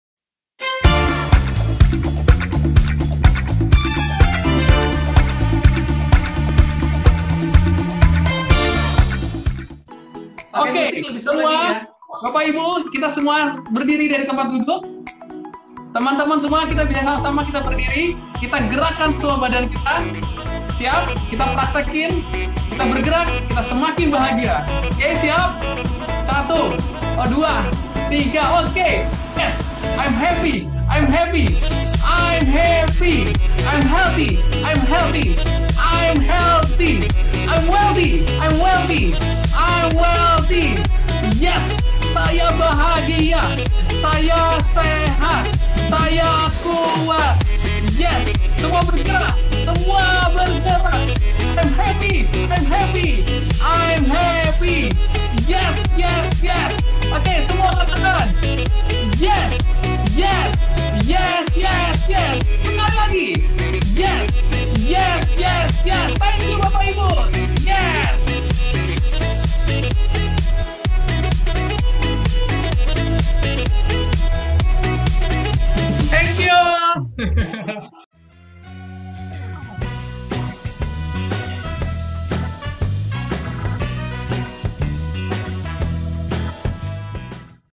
Senam untuk mengatasi Kepikunan (Dimentia)
Senam ini sangat bagus untuk mengatasi kepikunan (Dimentia)
bisa dilakukan setiap pagi